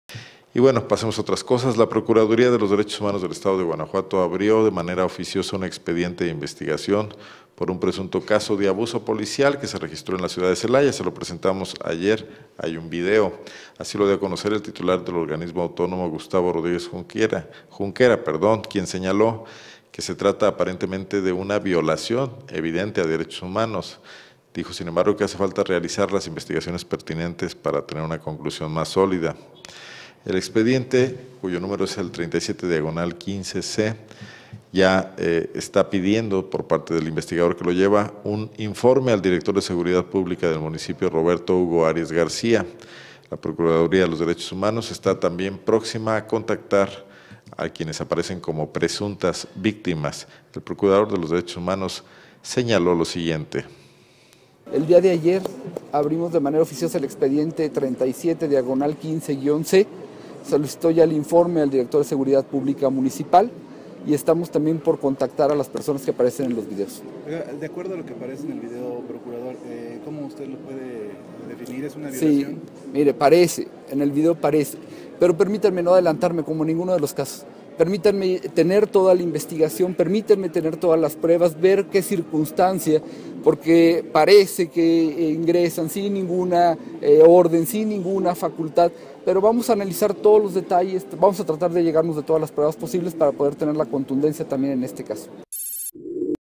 Abren PDEGH expediente por presunto abuso policial en Celaya
La Procuraduría de los Derechos Humanos del Estado de Guanajuato (PDHEG)  abrió de manera oficiosa un expediente de investigación por un presunto caso de abuso policial que se registró en la ciudad de Celaya.